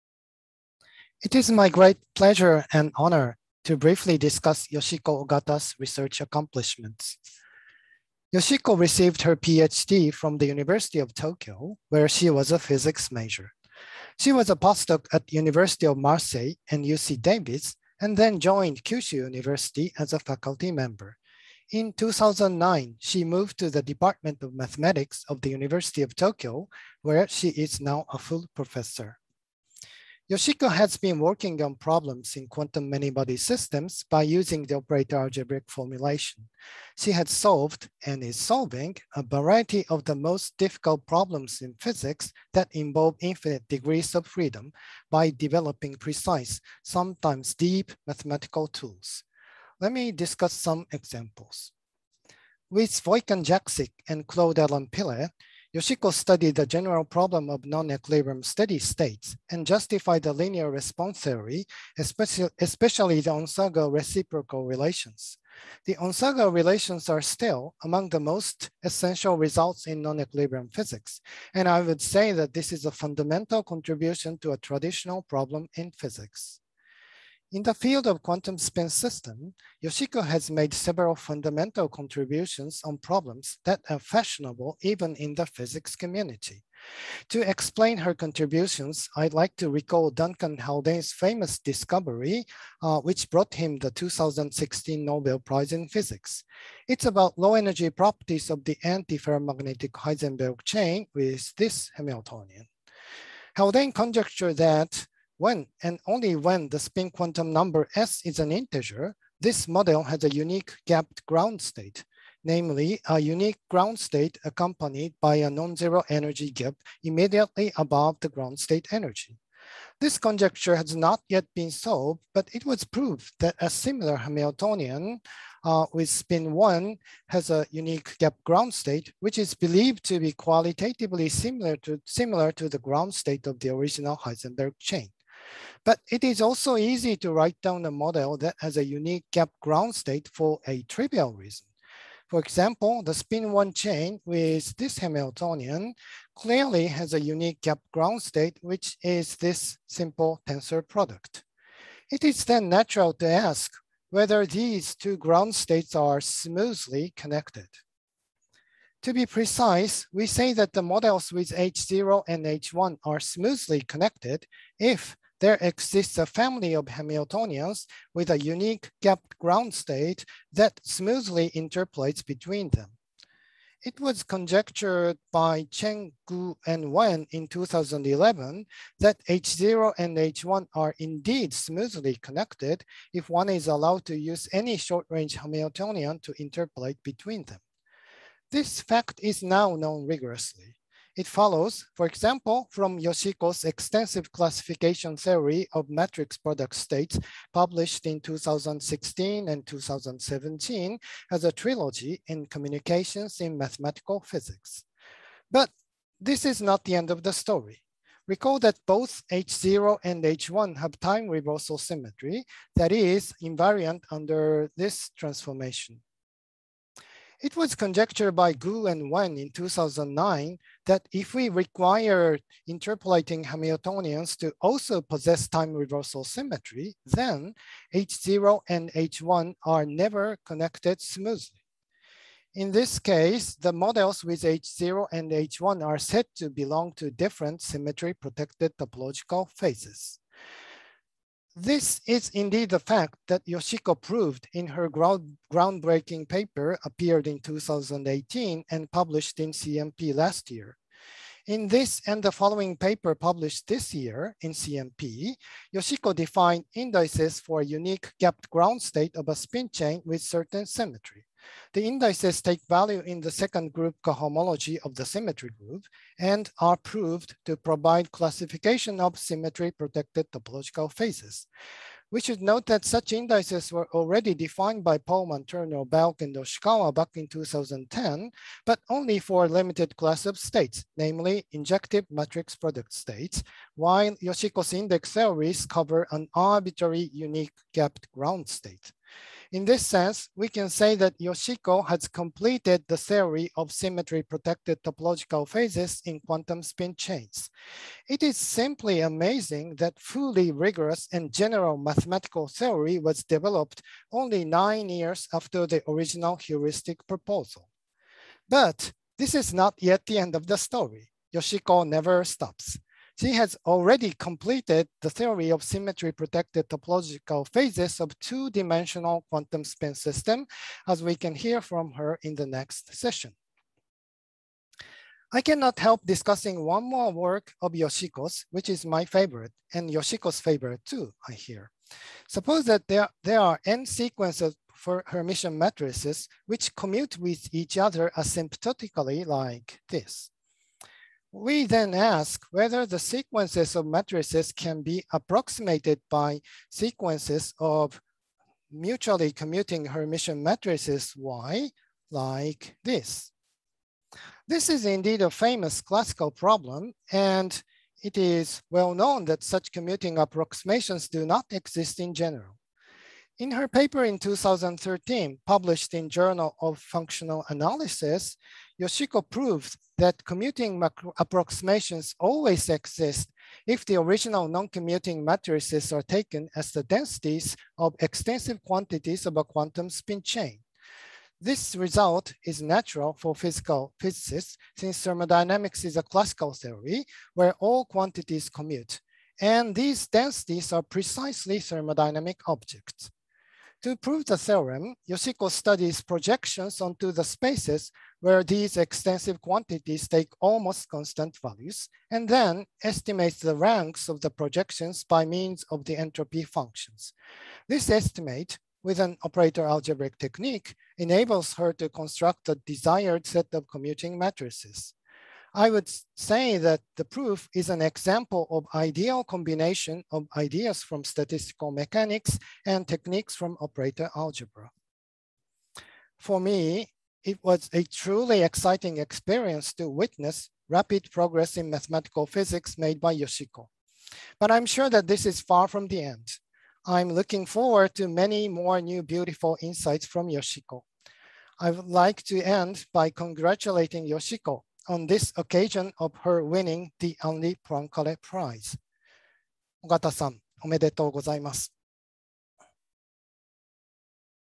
Laudatio for Yoshiko Ogata, Laureate of the Henri Poincare Prize 2021
Laudatio by Hal Tasaki for Yoshiko Ogata who received the Henri Poincare Prize 2021 on August 2, 2021.  The video was presented at the opening ceremony of International Congress of Mathematical Physics on August 2, 2021.